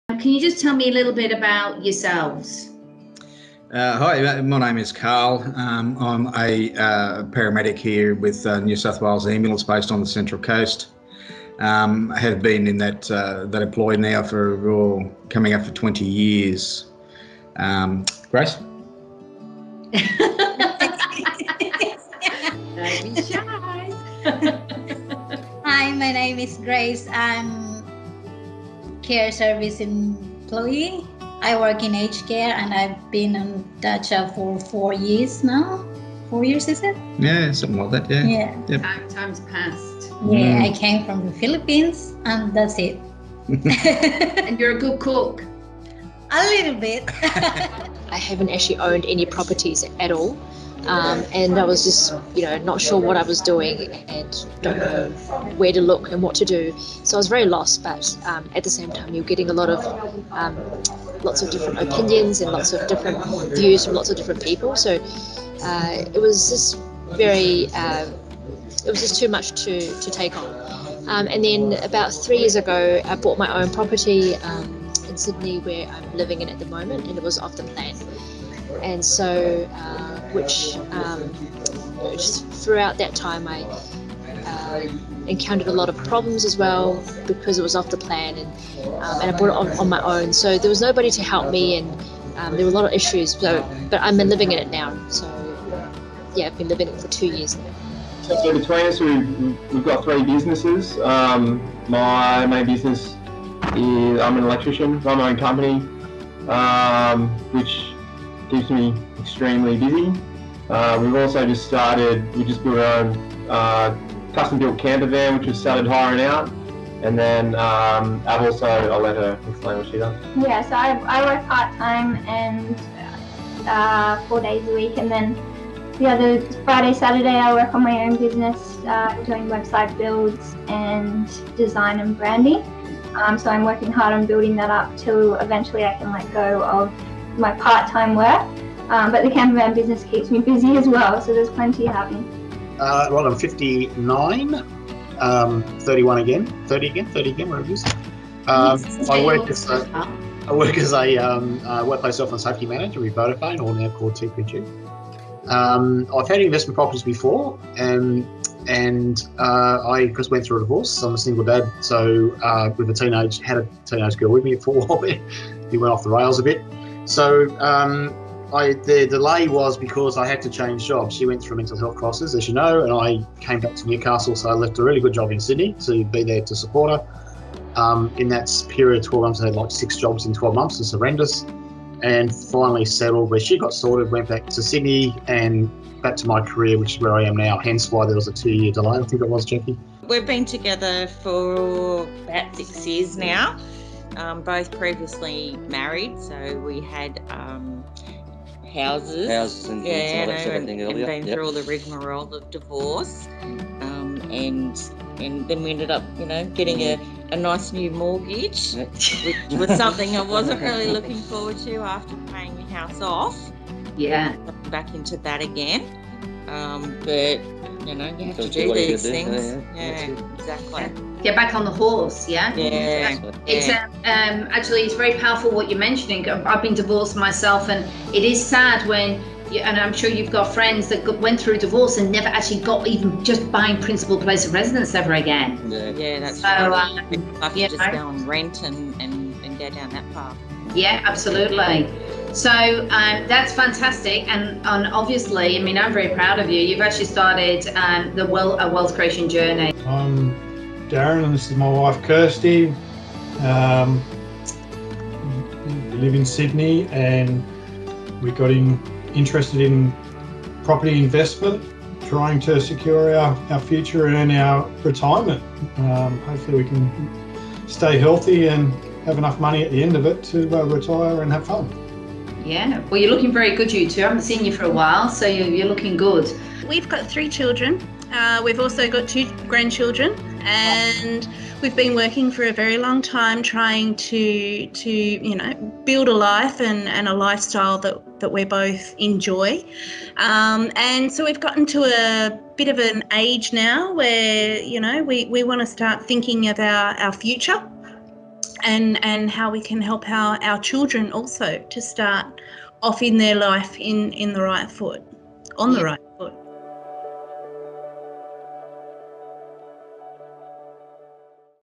Question Intro - Clients tell us a little bit about yourself!
We are very fortunate to have formed excellent partnerships with many of our clients. We have also formed true friendships. View our client testimonials.